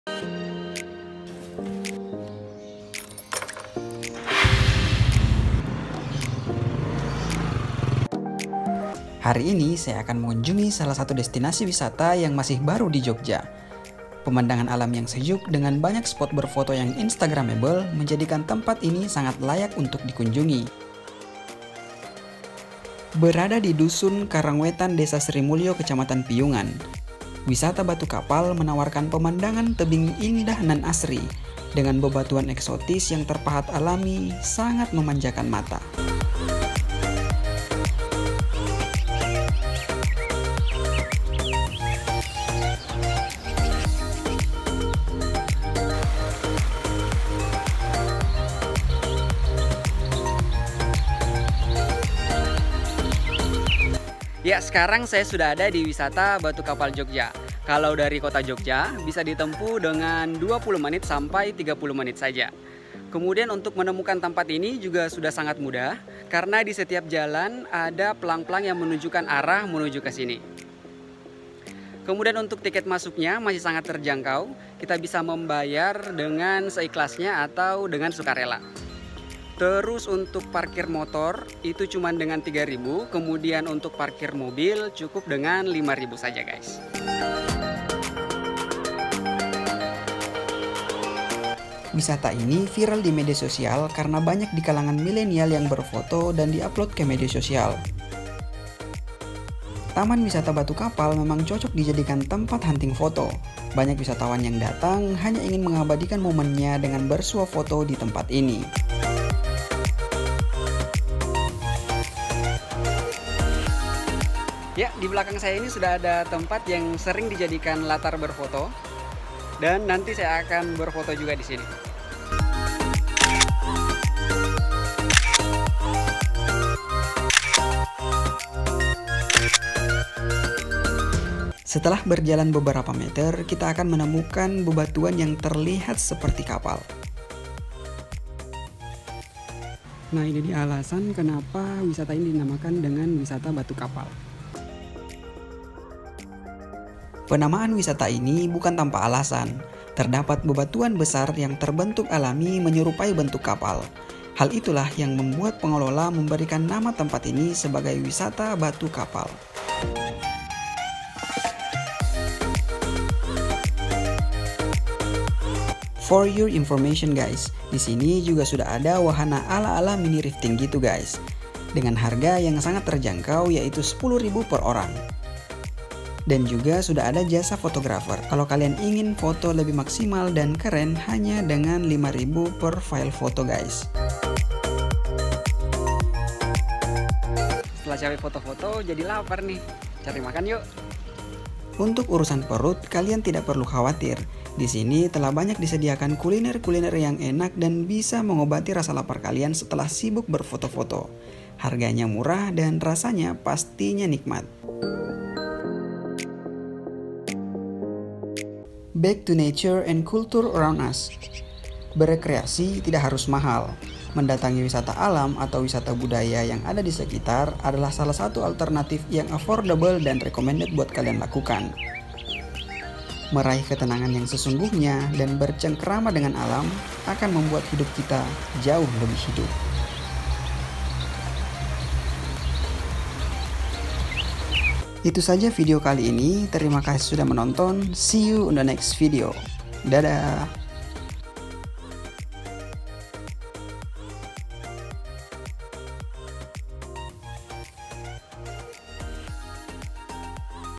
BATU KAPAL JOGJA I WISATA TERBARU DI JOGJA VIRAL❗
Ini dia wisata Batu Kapal Jogja yang lagi viral guys...
wajib banget kesini❗

Lokasi           : Desa Srimulyo, Kec. Piyungan, Bantul, Daerah Istimewa Yogyakarta 55792
Tiket masuk : seikhlasnya
Parkir            : motor Rp.3000
                        mobil Rp.5000
Rifting          : Rp.10.000 perorang
Foto              : Rp.5000 perfile

EQUIPMENT
🎥 HP IPHONE 6s